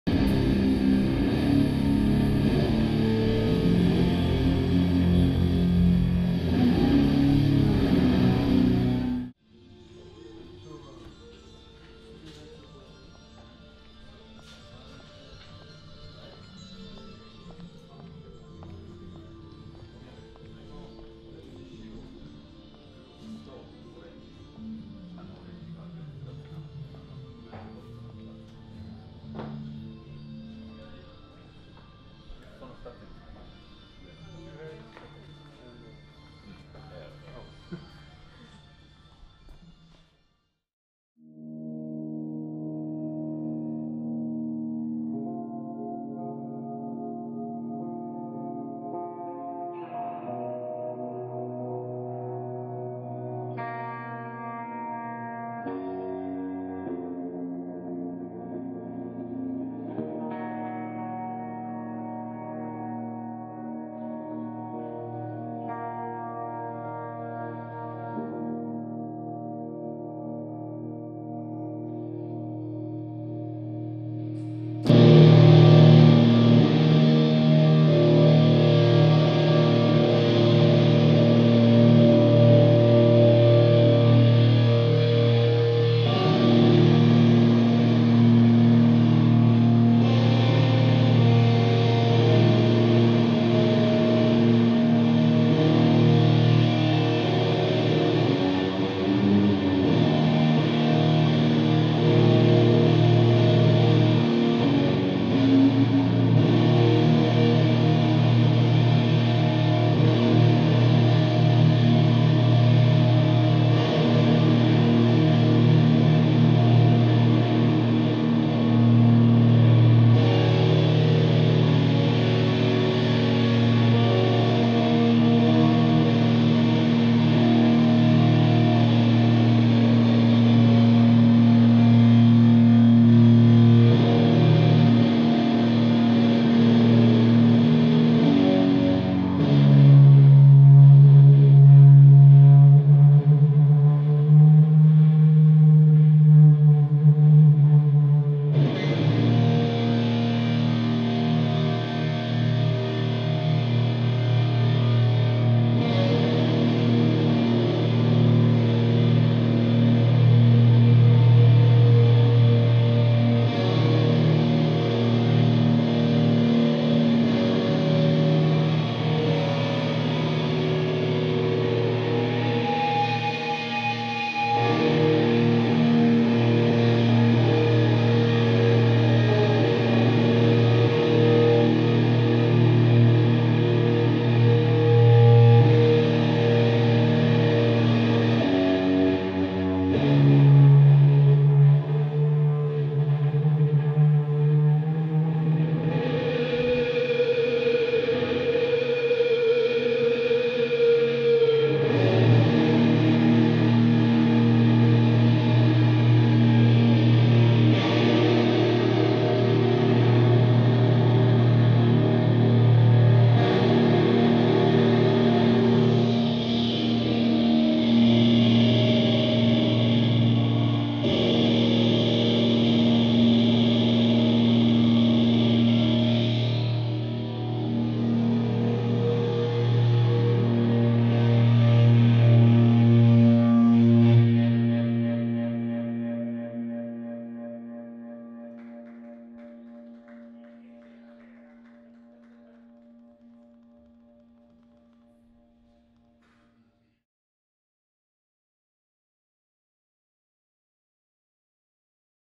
Wata from Boris - Surprise In-Store Solo Performance
The lovely Wata, guitarist for Japanese drone-rock band Boris, surprises shoppers at Criminal Records in Atlanta, GA, USA. Watch her make ears bleed when she plays full volume through a mountain of Orange amps and speaker cabs!